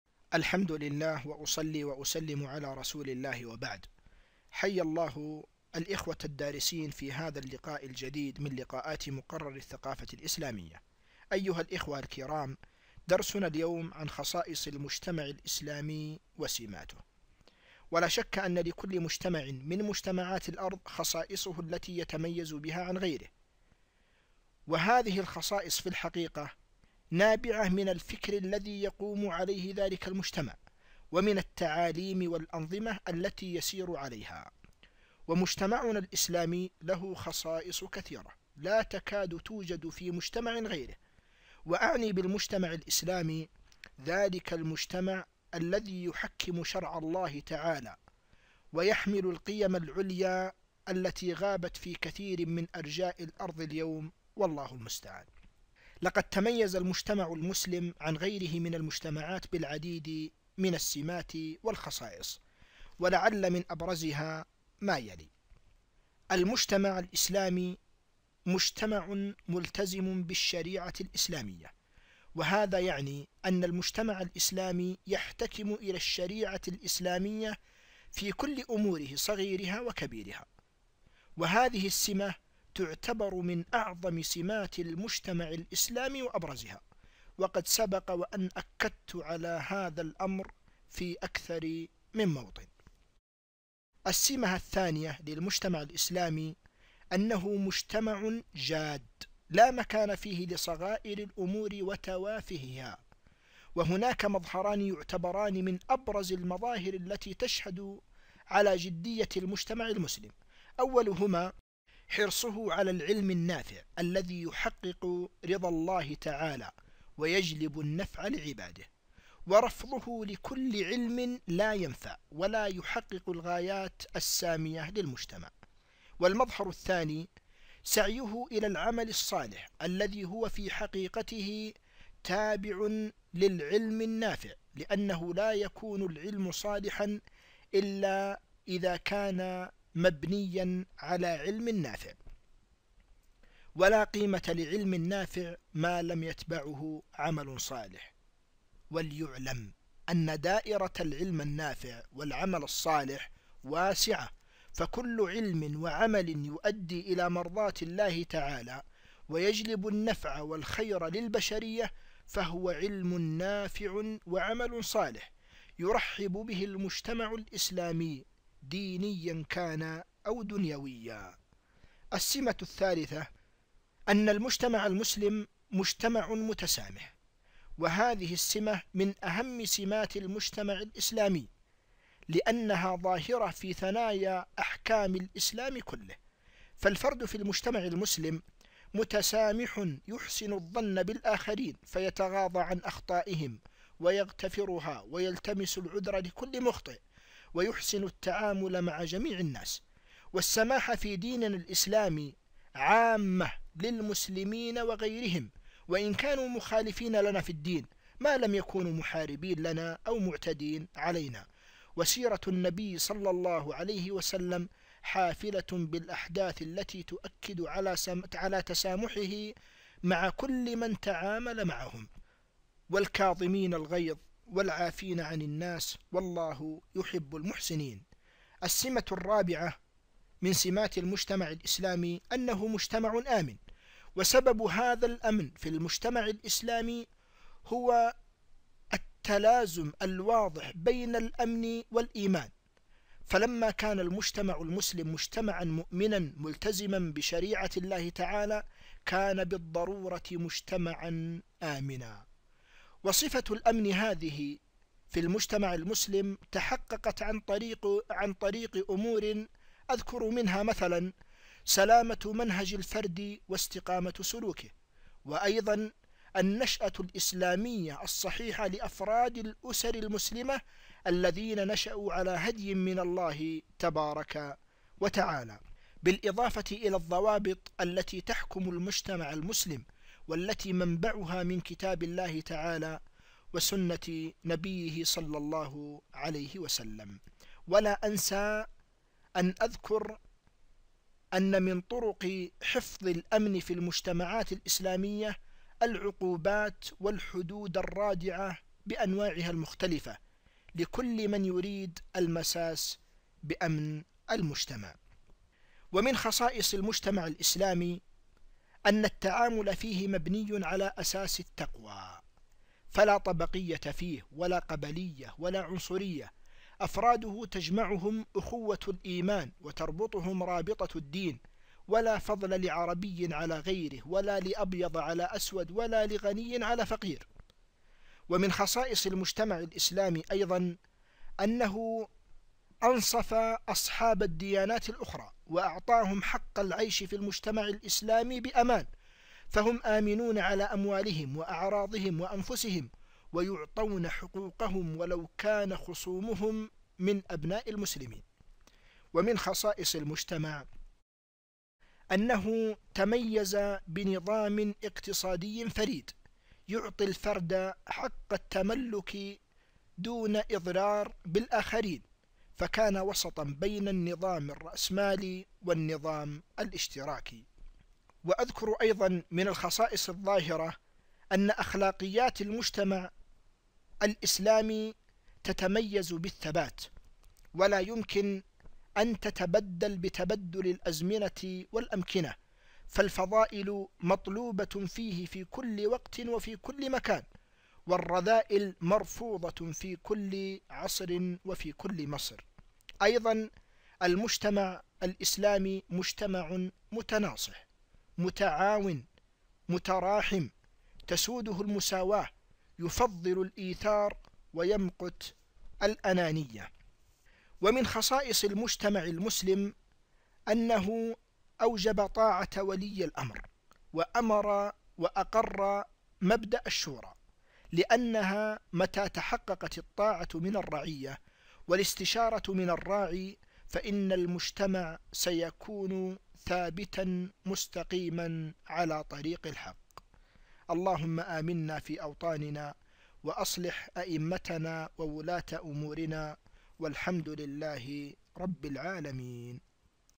خصائص المجتمع الإسلامي
أسباب تميز المجتمع الإسلامي عن غيره من المجتمعات.
ـ خصائص المجتمع الإسلامي: ( ملتزم بالشرع، جادّ، متسامح، آمن، متناصح، تسوده المساواة، متراحم، مطيع لولي الأمر )